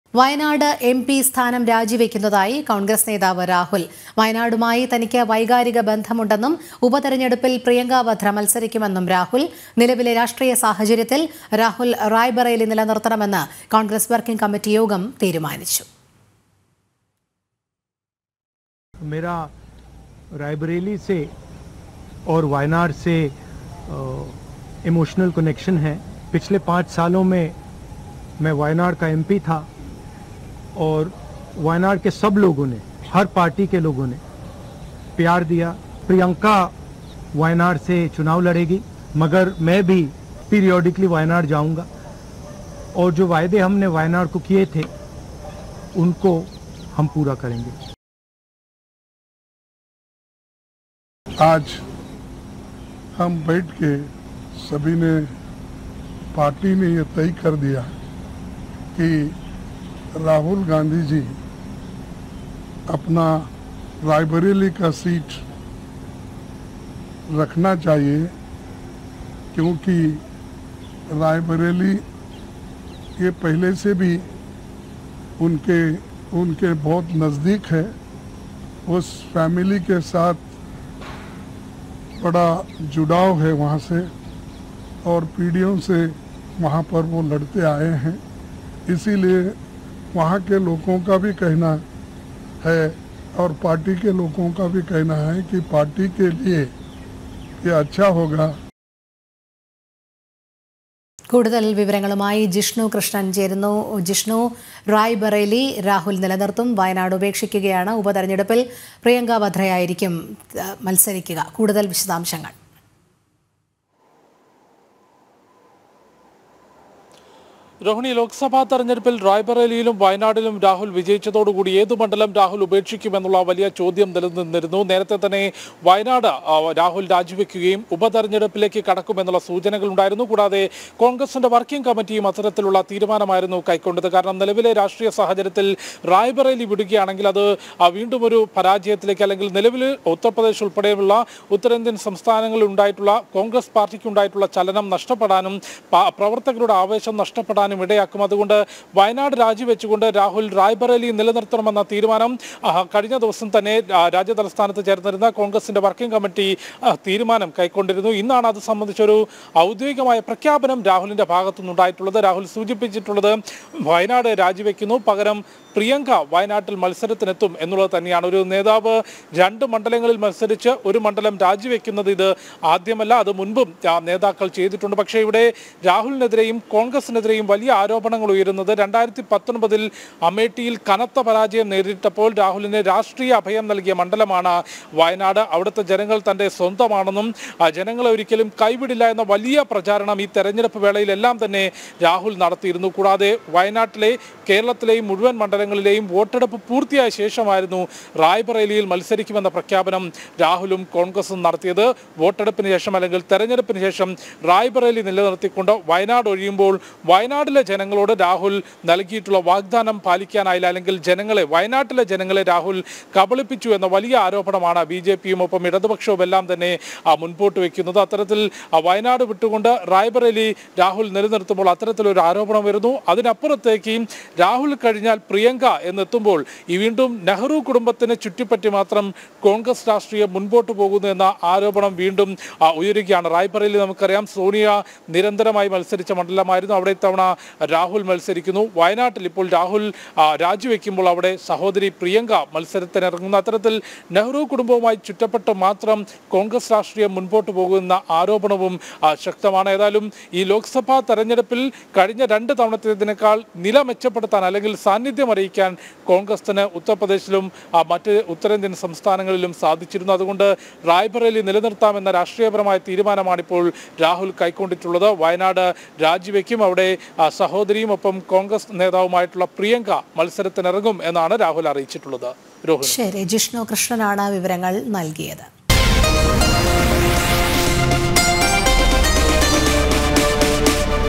വയനാട് എം പി സ്ഥാനം രാജിവെക്കുന്നതായി കോൺഗ്രസ് നേതാവ് രാഹുൽ
വയനാട് എം പി സ്ഥാനം രാജിവെക്കുന്നതായി കോൺഗ്രസ് നേതാവ് രാഹുൽ. വയനാടുമായി തനിക്ക് വൈകാരിക ബന്ധമുണ്ടെന്നും ഉപതെരഞ്ഞെടുപ്പിൽ പ്രിയങ്ക വദ്ര മത്സരിക്കുമെന്നും രാഹുൽ. നിലവിലെ രാഷ്ട്രീയ സാഹചര്യത്തിൽ രാഹുൽ  റായ്ബറേലി നിലനിർത്തണമെന്ന് കോൺഗ്രസ് വർക്കിംങ് കമ്മിറ്‍റി യോഗം തീരുമാനിച്ചു.